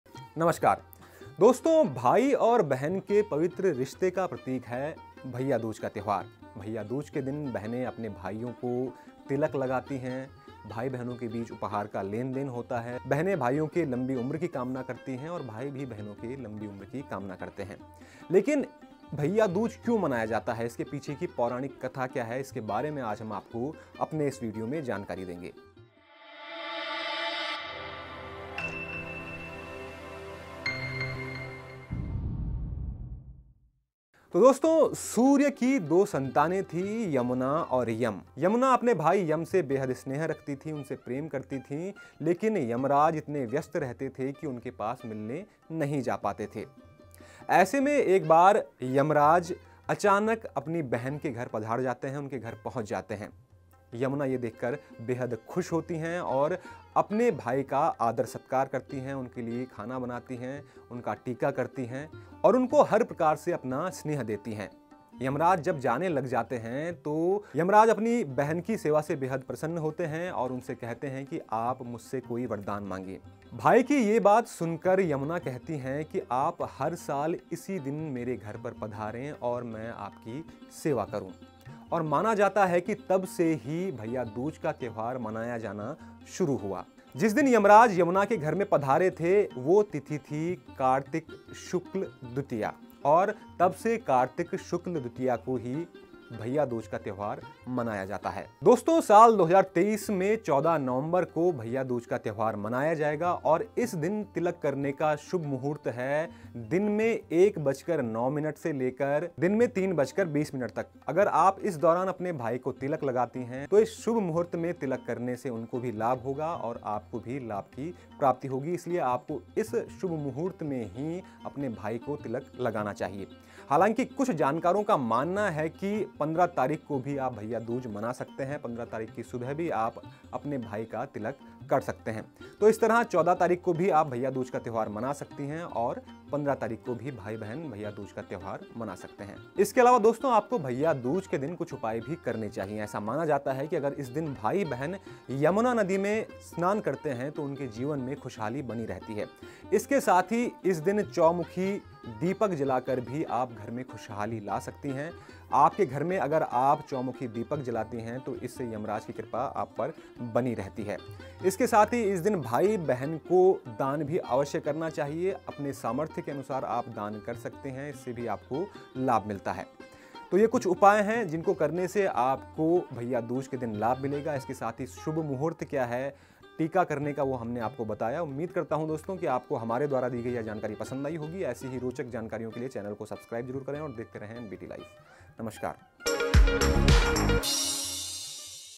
Bhai Dooj 2023: भाई दूज के दिन तिलक का शुभ मुहूर्त और पौराणिक कथा | NBT Life
Bhai Dooj 2023: भाई दूज को बहन-भाई के पवित्र रिश्ते का प्रतीक माना जाता है। साल 2023 में भाई दूज का त्योहार 14 नवंबर को मनाया जाएगा। इस दिन तिलक करने का शुभ मुहूर्त कब है और इस दिन से जुड़ी पौराणिक कथा क्या है इसके बारे में आज हम आपको अपने इस वीडियो में जानकारी देंगे।